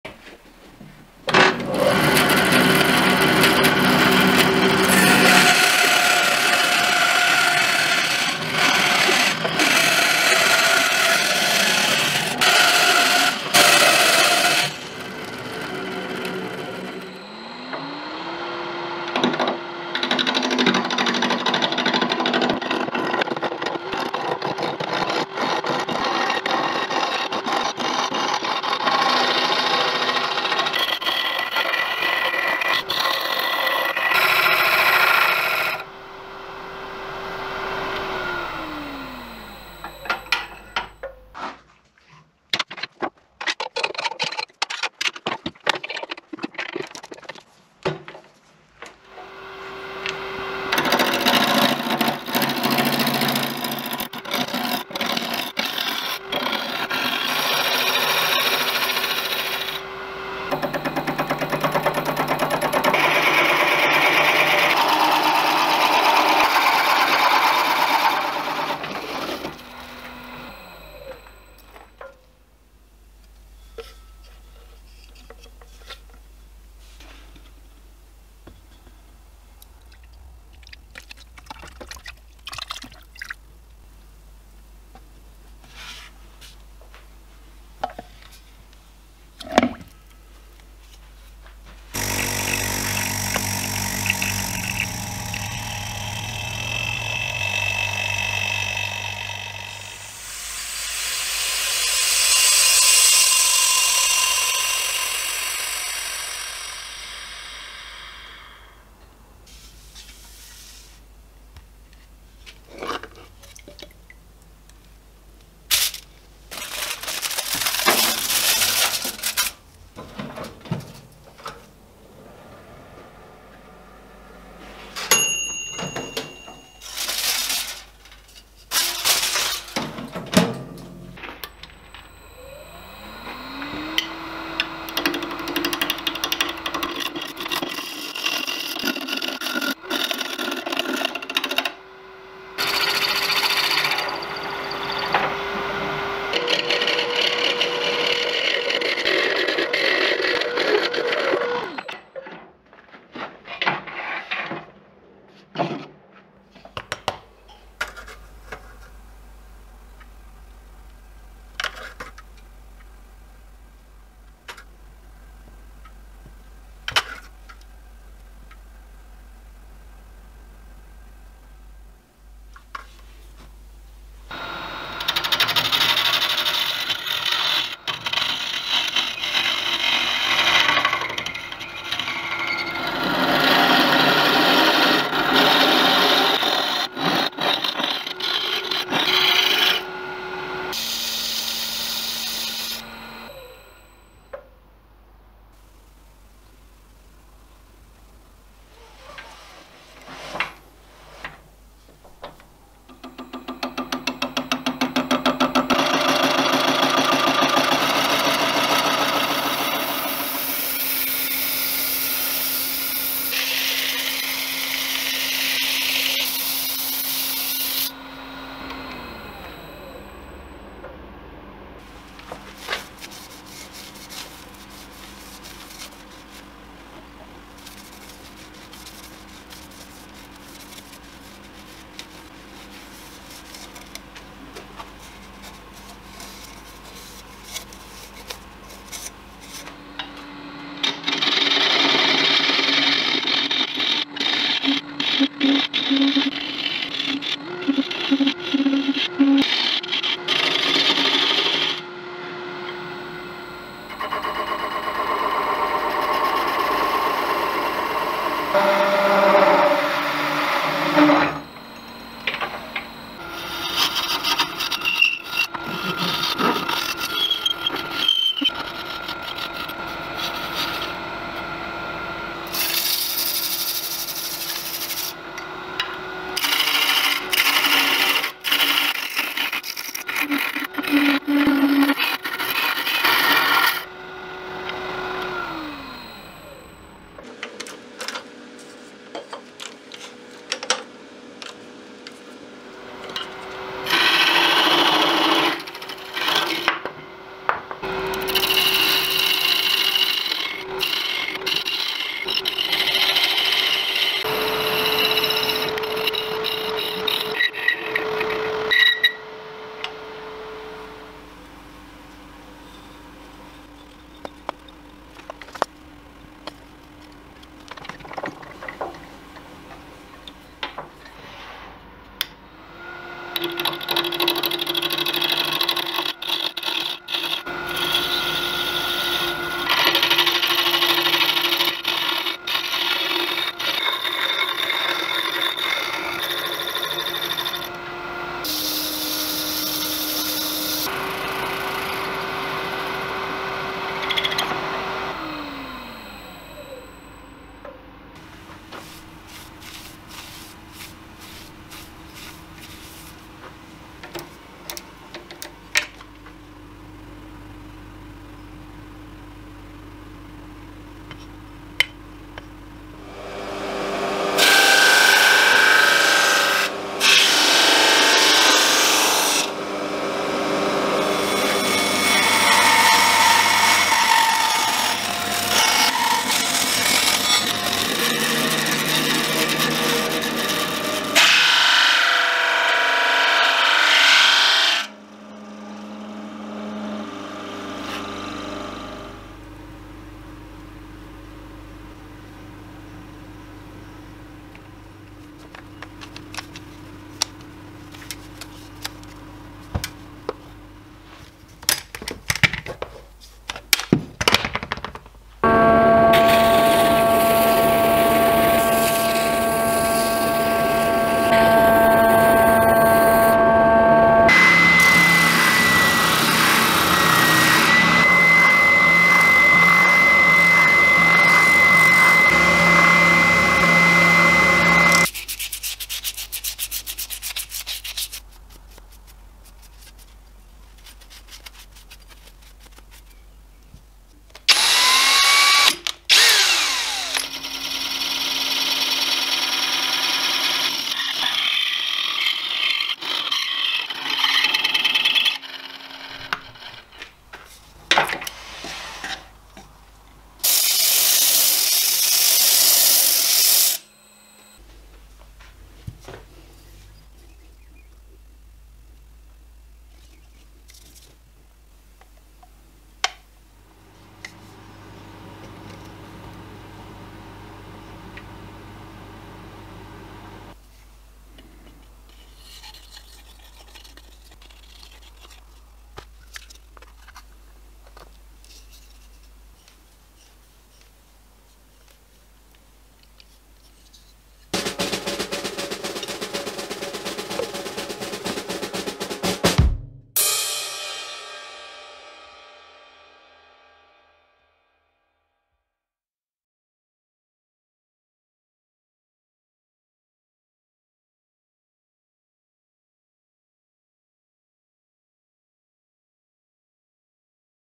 Woodturning - The French Press / Cafetiere
Main piece of the Cafetiere is made from a piece of Applewood, plenty of Burl in this piece and it really pops once the Danish oil is applied. Metal is Pewter.